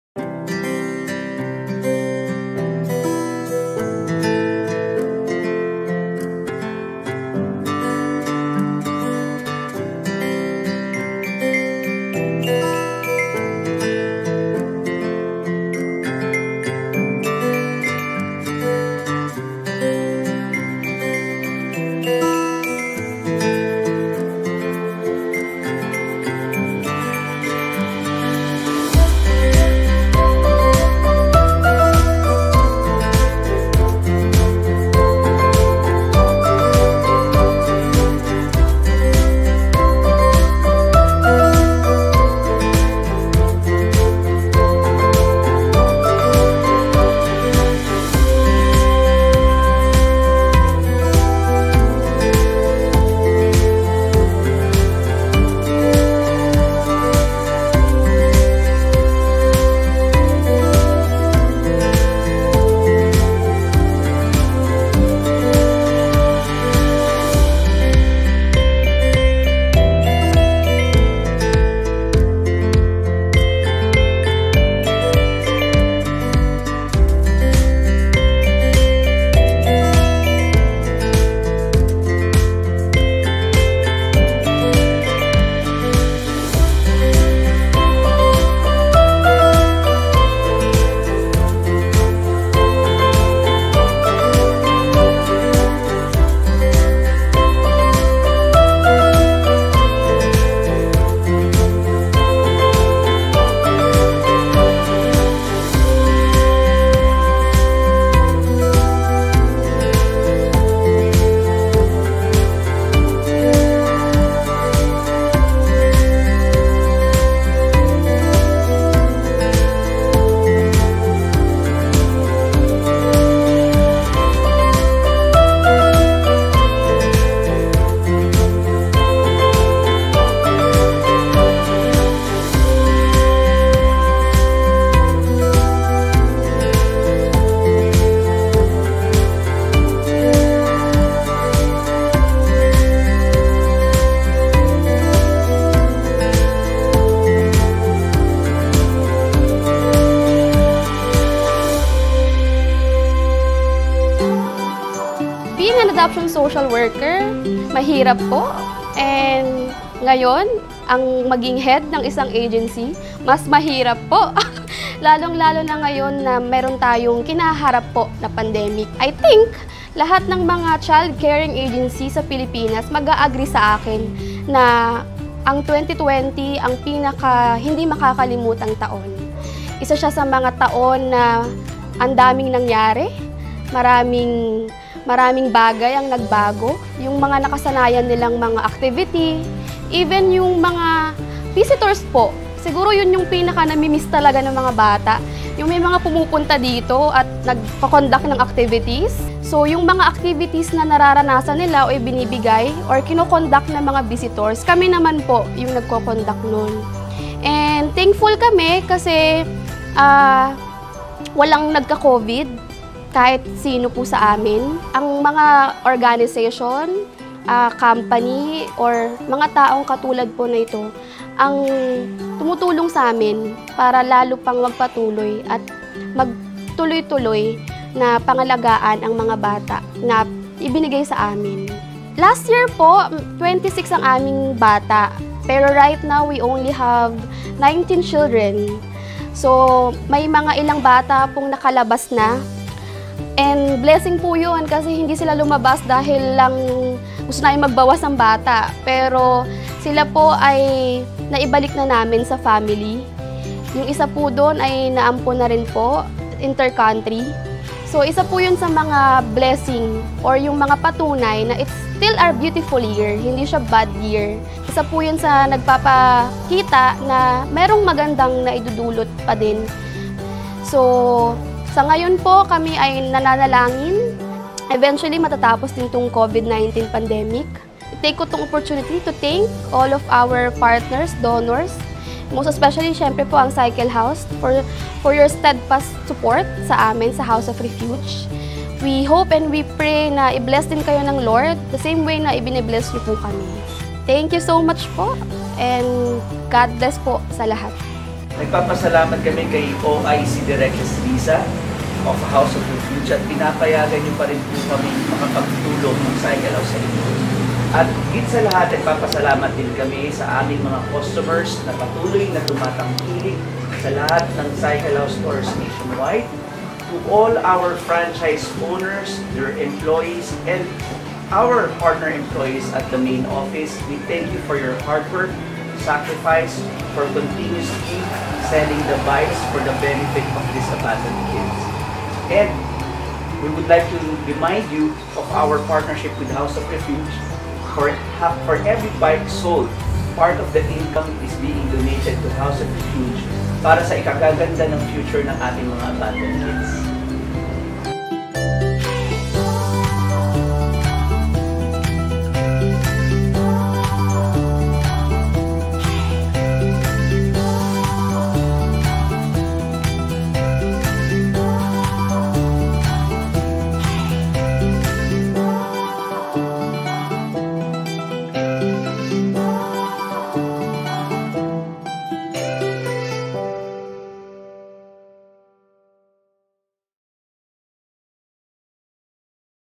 Help even in small ways!
Our corporate social responsibility at CycleHouse. We will continue to support the House of Refuge Foundation because we believe that even in small things, we can make a difference.

For every bike sold, part of our income will be donated to them to help have a better future. 🚲🚲

We would like to thank our customers, partner employees and franchisees for responding the needs and challenges for the last two years.